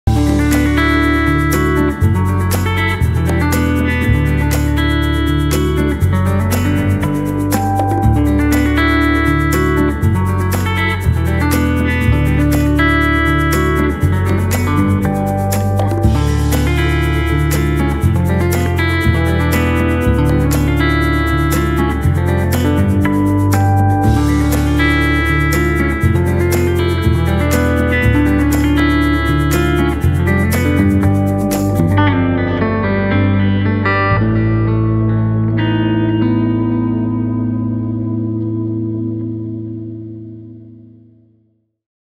Deluxe Dulcimer Hammer ons
Dulcimer hammer ons. Feature the bridge pickup on the 3G Deluxe.